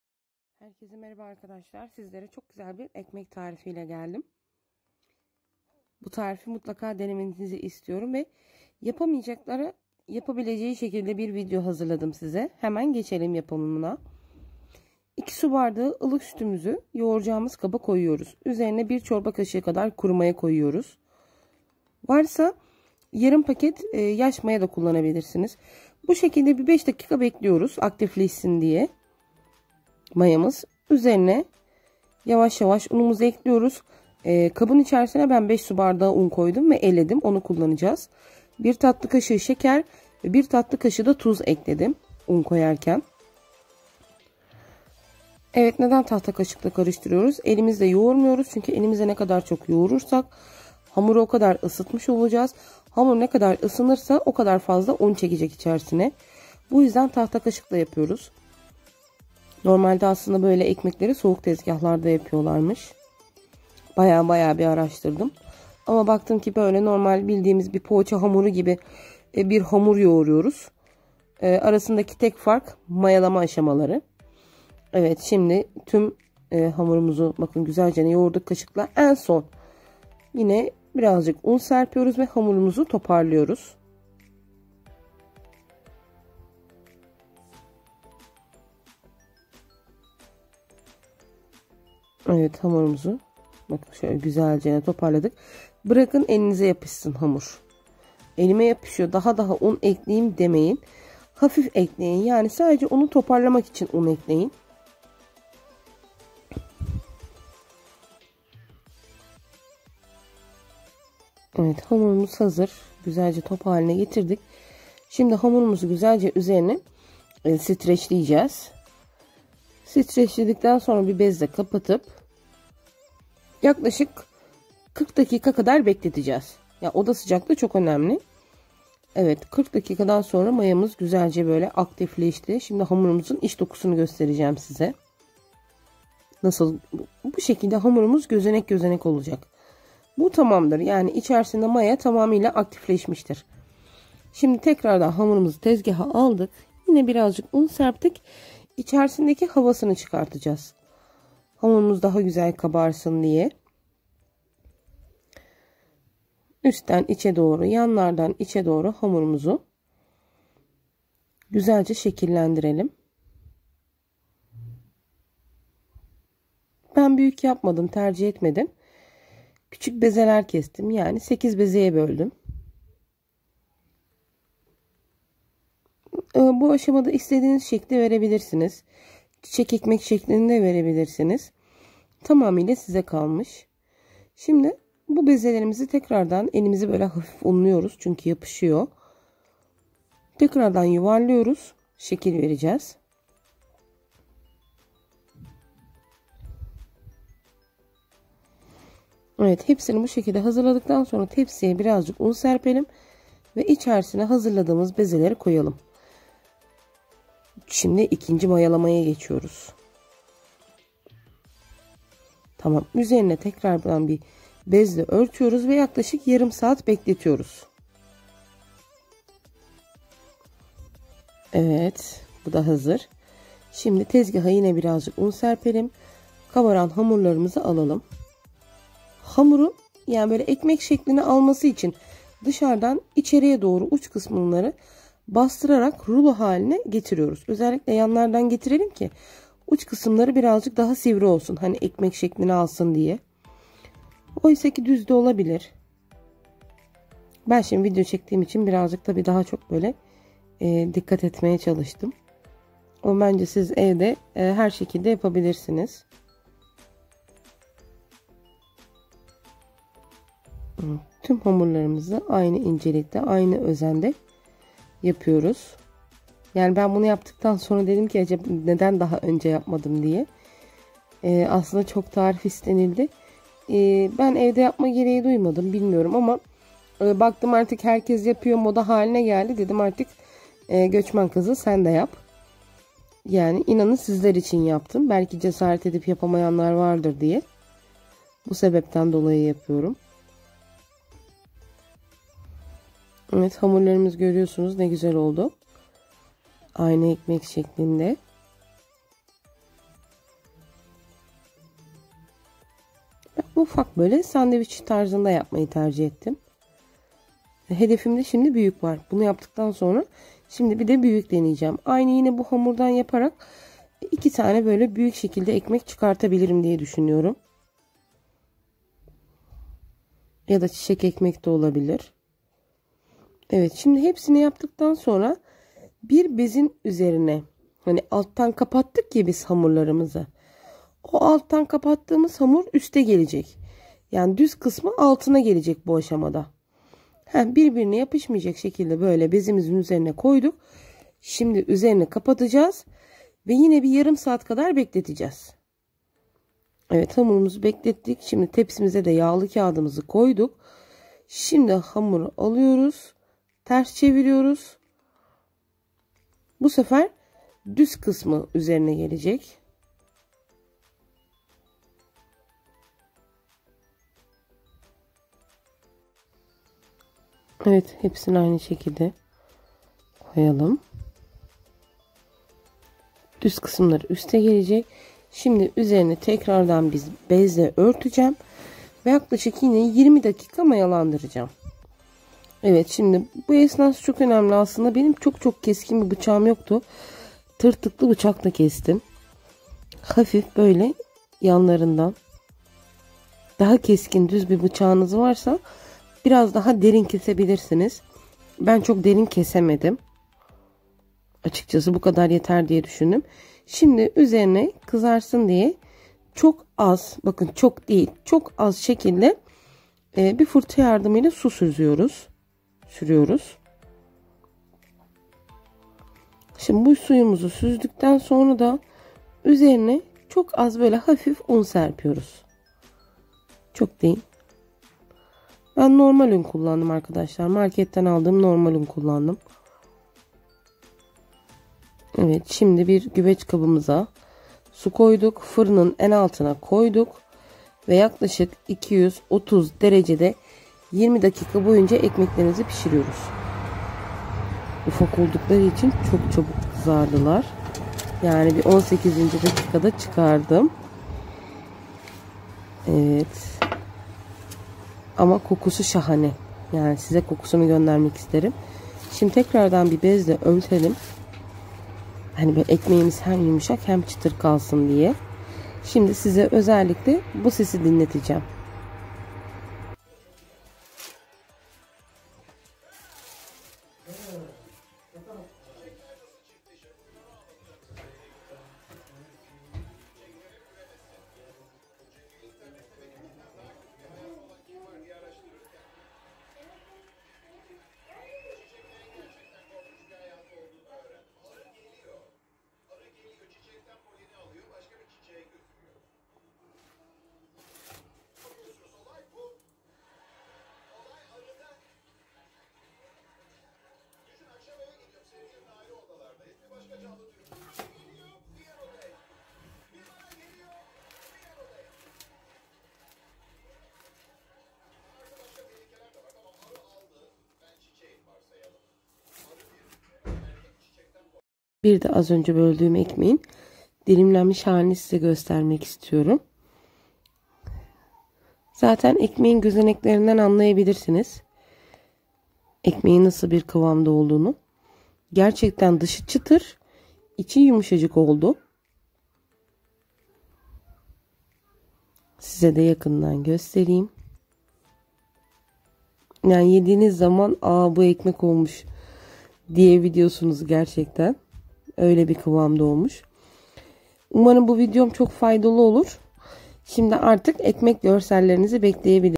🥖EKMEK YAPMAYAN KALMASIN🥰🥖COK KOLAY ANLATIMLI MULEMMEL BIR TARIF🥖🥖
EV YAPIMI EKMEK TARIFI
MALZEMELER
*2 su bardağı ılık süt
*1 paket instant maya
(10gr =1 corba kasıgi )
*1 tatlı kaşığı tuz
*1 tatlı kaşığı şeker
*5 su bardağı elenmiş un
YAPILISI
Yoğurma kabına sutu ekleyelim uzerine luru mayayi ekleyelim ve 3 dk bekletelim maya aktiflessin.Ardindan yavas yavas unumuzu ekleyelim bu esnada el ile degil tahta kasik ile yoguralim unu eklerken seker ve tuzuda ekleyelim.Tahta kasikla toparlanamayacak hale geldiginde simdi elimizle hamuru toparlayalim hamur ele yapısabilir sorun etmeyin.Hamuru guzelce toparladik uzerini strecledik ve 40 d beklettik ardindan bekleyen hamuru az un serpilmis tezgahta yoguralim havasi ciksin sonra 8 esit bezeye bolelim buyukte yapabilirsiniz ben ufak yaptim.Bu bezeleride yuvarlayalim un serptigimiz tepside 30 dk uzeri kapali bekletelim.Beklettikten tezgaha biraz un serpelim hamuru hafif elimizle bastiralim ve distan ice dogru uc kısımları bastıra bastira rulo haline getirelim.Ekmekler oval sekli alicak daha sonra yine bir bezin uzerine koyalim ve uzerini kapatalim 20-30 dk tekrar mayalandiralim.Ardindan tepsiye alalim.tepsiye alirken birlestirdigimiz kisim alt tarafa gelicek.10 dk tepside bekletelim ve kenarlarindan cizik atalim ne kadar derin cizik atarsaniz o kadar iyi ben cok icten kesemedim.Daha sonra firca ile uzerine su surelim ama cok az ve suzgecle hafif un serpelim onceden isitilmis 200°firinda 20 dk pisirelim.Firina atarken firinin alt katina bir kaseye su ekleyelim ekmegimiz yumusak olsum diye..Yapicaklara simdiden kolay gelsin..

Müzisyen: @iksonofficial